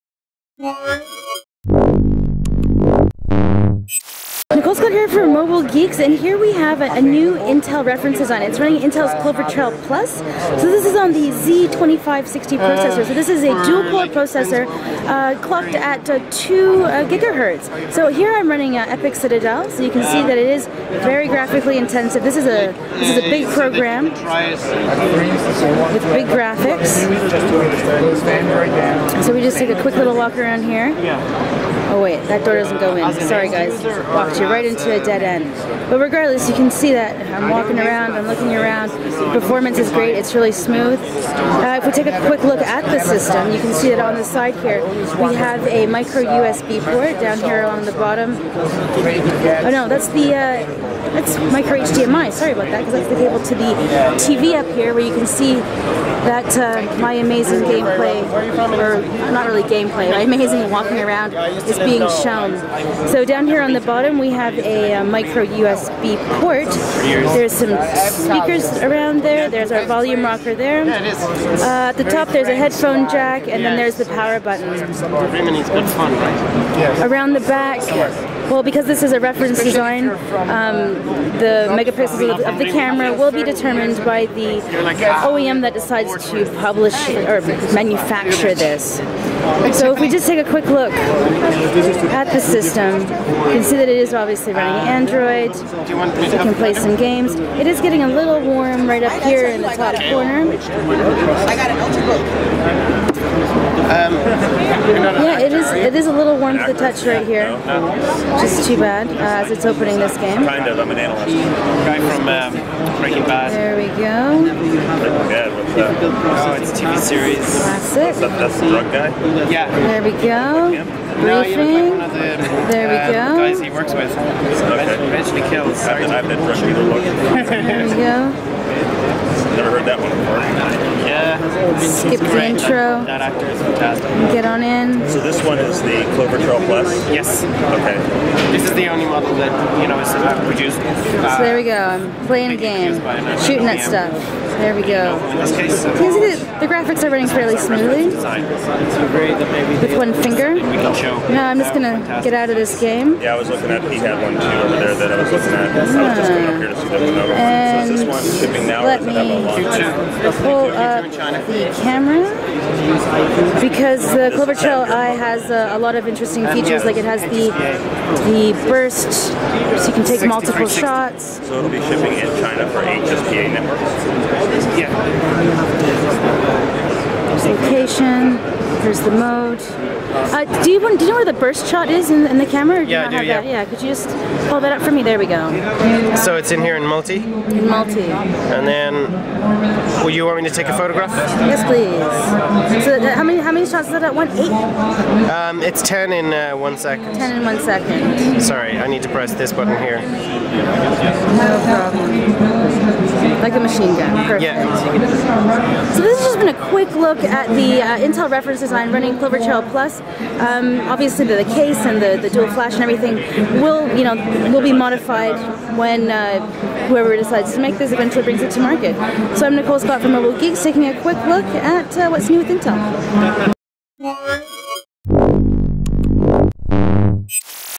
Intel Clover Trail+ Atom Z2580 Epic Citadel Graphics Test & Reference Design Hands On
intel clover trail+ z2560 The SoC also includes an Intel Graphics Media Accelerator engine clocked at up to 533MHz with boost mode, and delivering up to three times the graphics performance in 3D graphics apps compared to predecessor as well as full 1080p hardware-accelerated video encode and decode at 30fps. The GPU supports 1900x1200 maximum screen resolution, hence it will hardly be able to power leading-edge Android-based tablets.